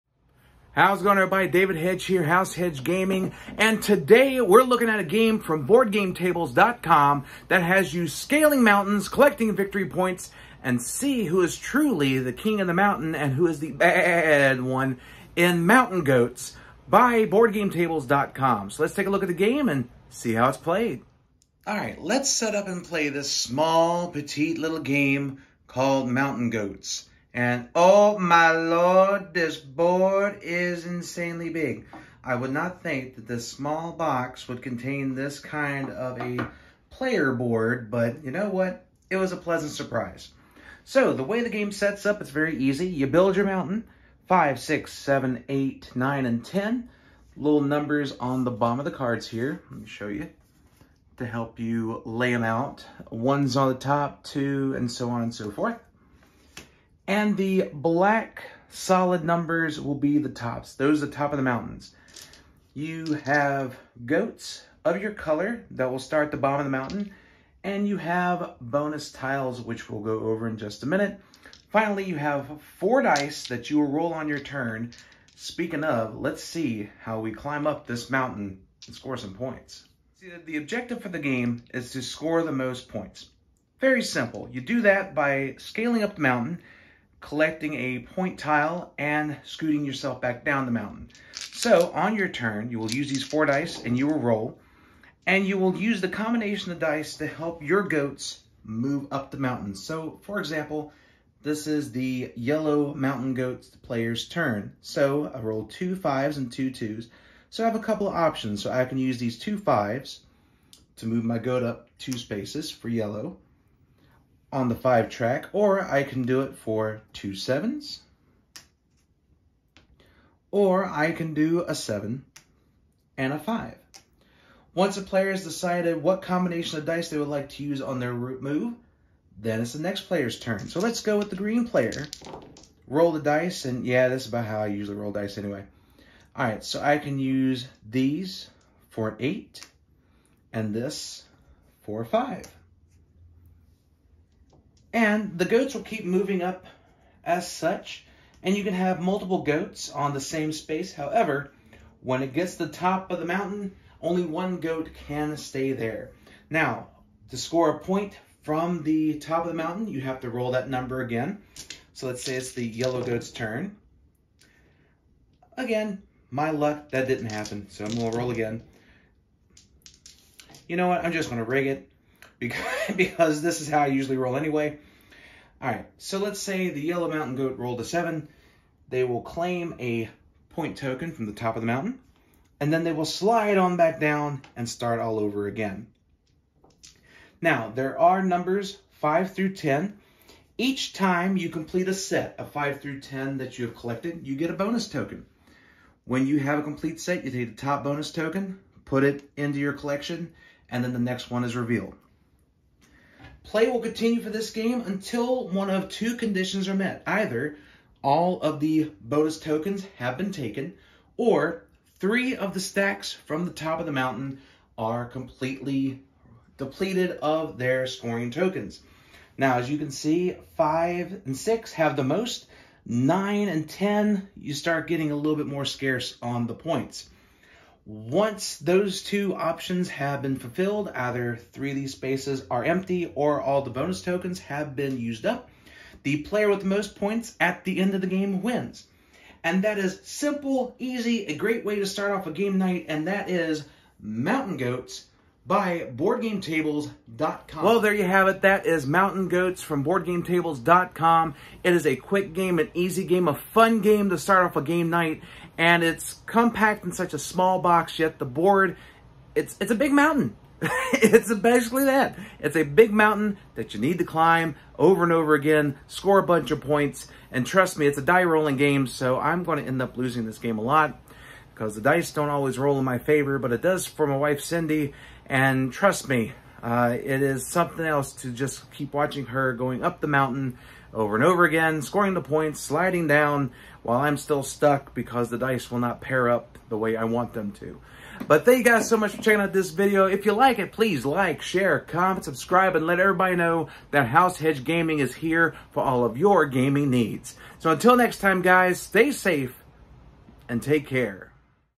How to Play! Mountain Goats - (Tutorial and Review)
Want a quick game to kick off your next game night? Join me as I check out Mountain Goats from BoardGameTables.com. One of the latest additions to our gaming collection from Gen Con.

Like the video? Make sure to leave a comment in below.

Follow on Facebook: Facebook.com/househedgegaming